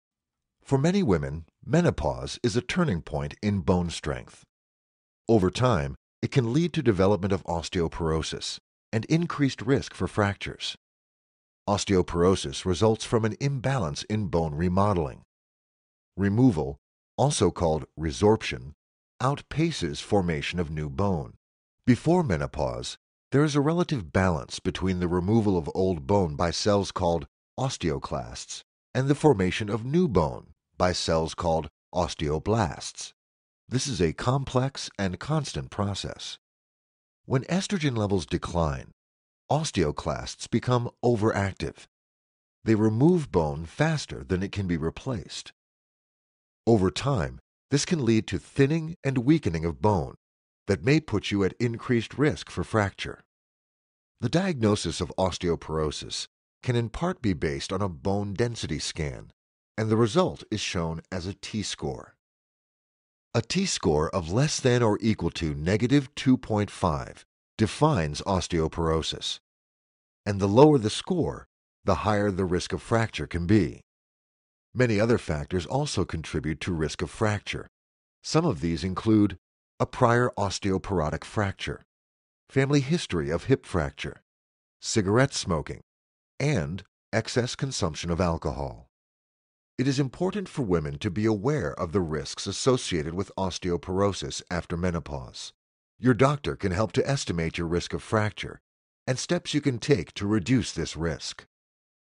About Osteoporosis and Fractures After Menopause
Para muchas mujeres, la menopausia es un punto de inflexión en la resistencia ósea. Con el tiempo, puede conducir al desarrollo de la osteoporosis y aumento del riesgo de fracturas. La osteoporosis resulta de un desequilibrio en la remodelación ósea. Remoción - también llamado reabsorción - la formación de nuevo hueso supera. Antes de la menopausia, hay un relativo equilibrio entre la eliminación de hueso viejo por las células llamadas osteoclastos y la formación de nuevo hueso por las células llamadas osteoblastos. Este es un proceso complejo y constante. Cuando los niveles de estrógeno disminuyen, los osteoclastos se vuelven hiperactivas. Ellos quitan los huesos más rápido de lo que puede ser reemplazado. Con el tiempo, esto puede llevar a un adelgazamiento y debilitamiento de los huesos que lo pueda poner en riesgo de fractura. El diagnóstico de la osteoporosis en parte puede estar basada en una exploración de la densidad ósea y el resultado se muestra como una puntuación T. Una puntuación T de menor o igual a negativo 2.5 define la osteoporosis y el más bajo es el puntaje, mayor es el riesgo de fractura puede ser. Muchos otros factores también contribuyen al riesgo de fractura. Algunos de estos incluyen: Una fractura osteoporótica previa, antecedentes familiares de fractura de cadera, hábito de fumar y consumo excesivo de alcohol. Es importante que las mujeres sean conscientes de los riesgos asociados con la osteoporosis después de la menopausia. Su médico le puede ayudar a estimar el riesgo de fractura y los pasos que puede tomar para reducir este riesgo.